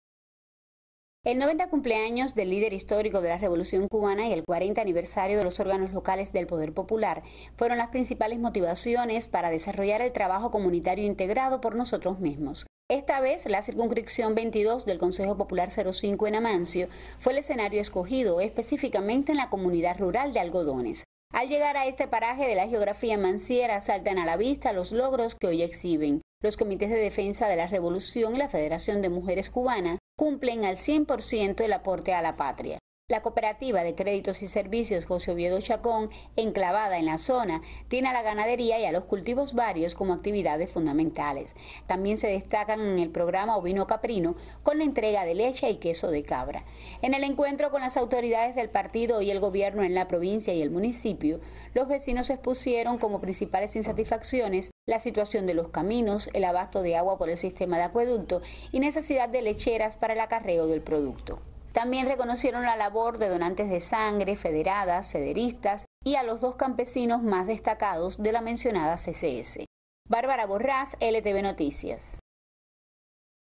Con la mira puesta en las comunidades
La comunidad rural de Algodones fue el escenario para la evaluación de las tareas del Trabajo Comunitario Integrado “Por Nosotros Mismos”.